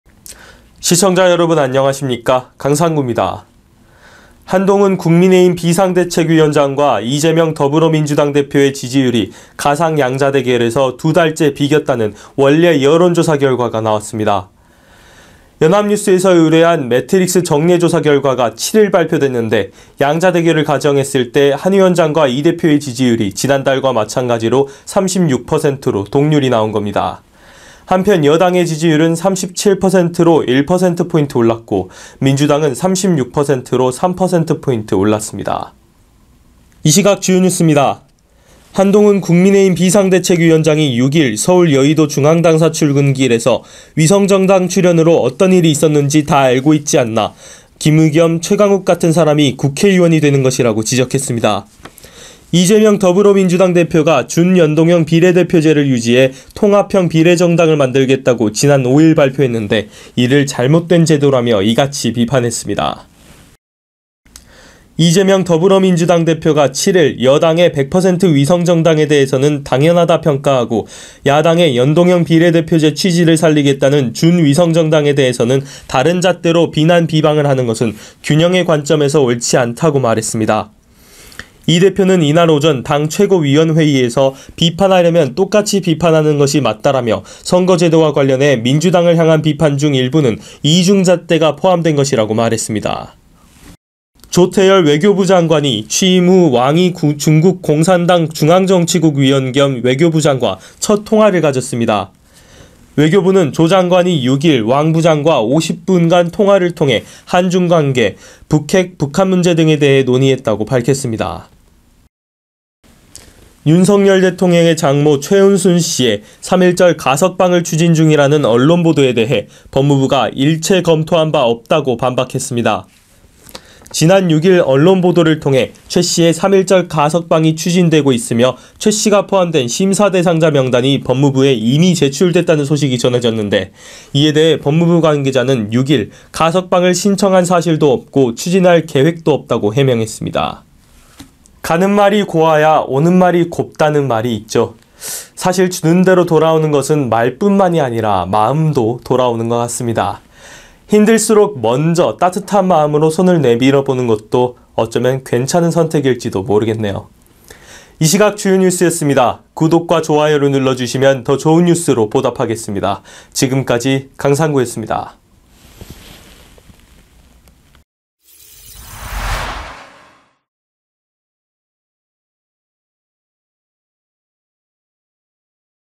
이 시각 주요 뉴스_ 2024년 2월 7일
1. 한동훈 "위성정당, 김의겸·최강욱 같은 국회의원 나온 이유"
2. 이재명 "여야 위성정당 비판, 이중잣대"
3. 조태열, 왕이 중국 외교부장과 북핵·탈북민 문제 논의
4. 법무부 "대통령 장모 가석방, 검토한 적 없다"

진행: 강상구 앵커
영상: 김현우 기자